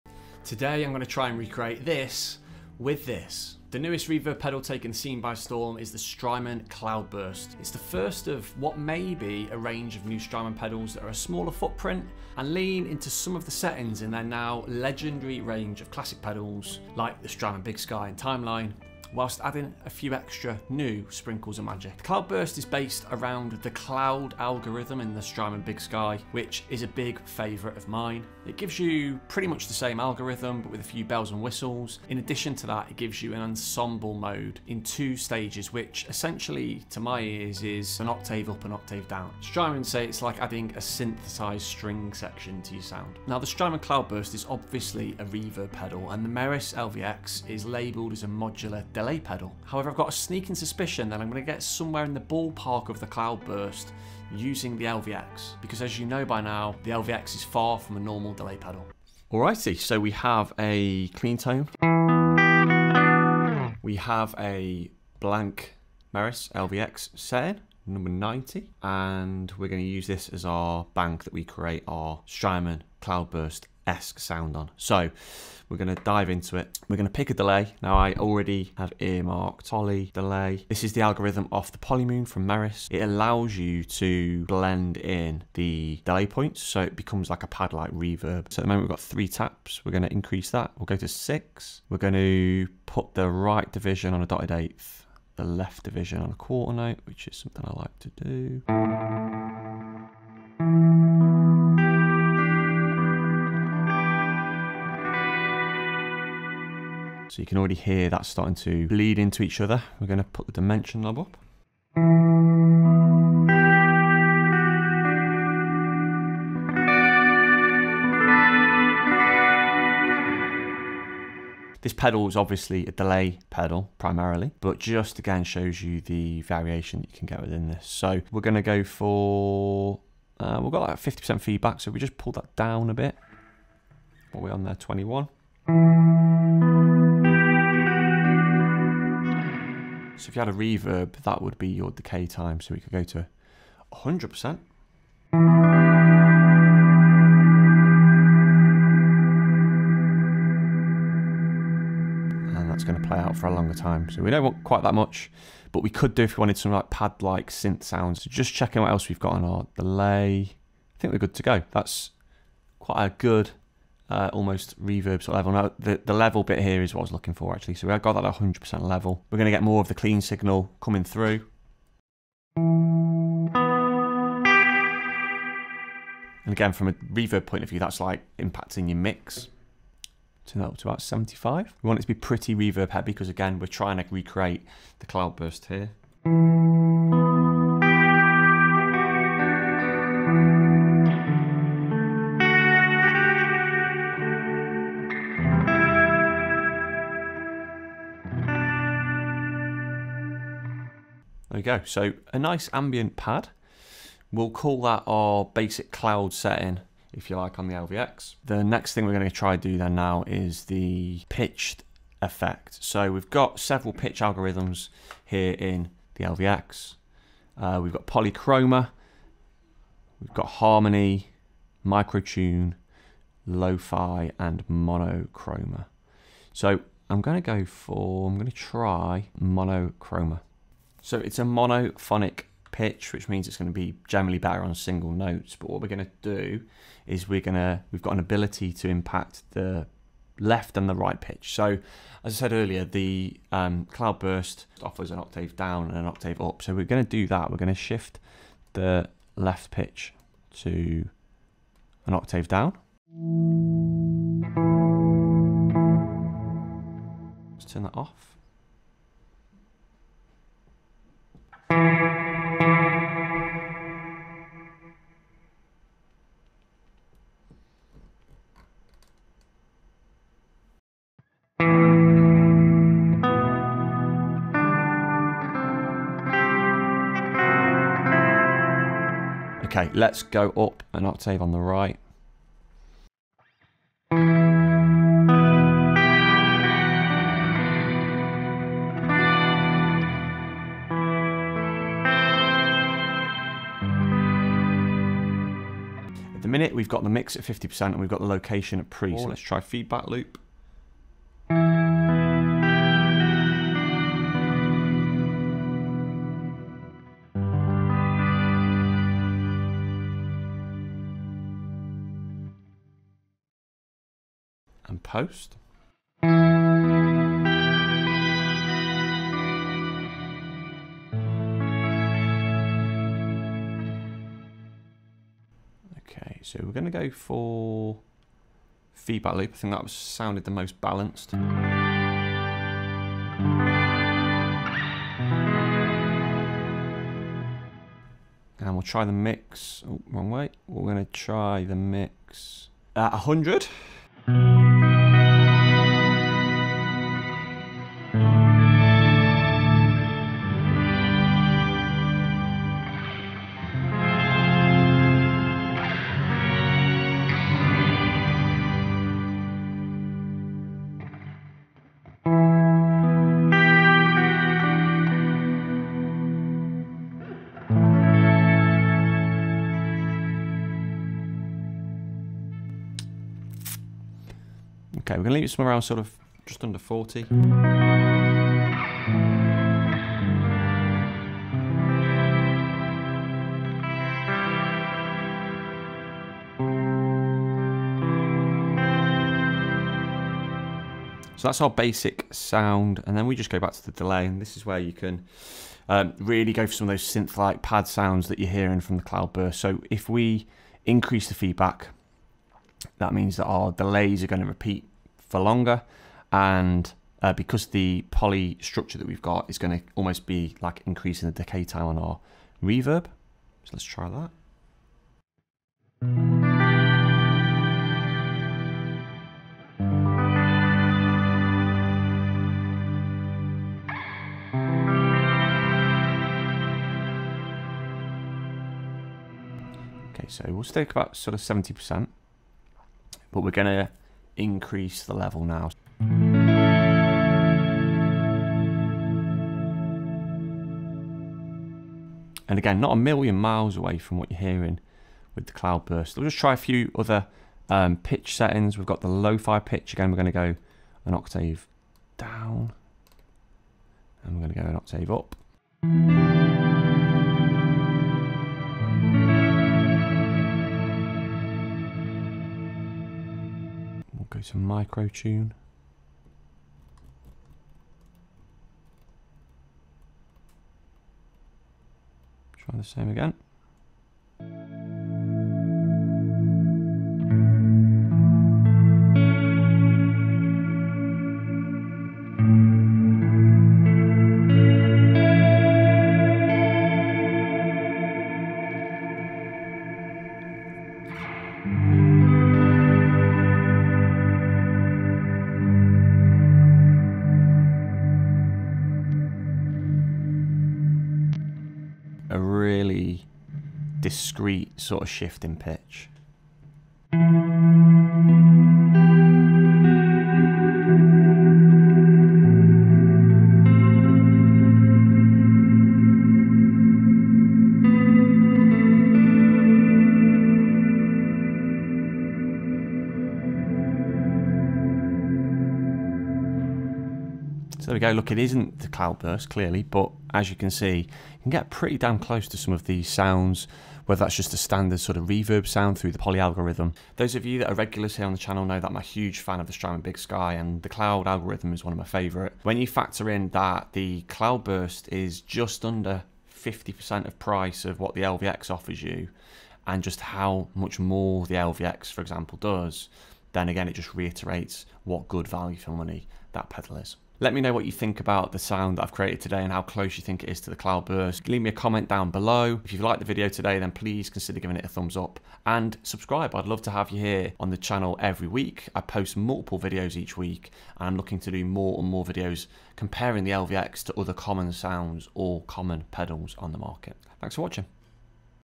Can the Meris LVX recreate the Strymon Cloudburst Pedal?
Can the Meris LVX recreate the Strymon Cloudburst Pedal?  Im on a mission to see how far the Meris LVX can go and in this video I tried to recreate some of the sounds from the Strymon Cloudburst.  What pedal/sound should I try and replicate next?

All content created by Nick Rees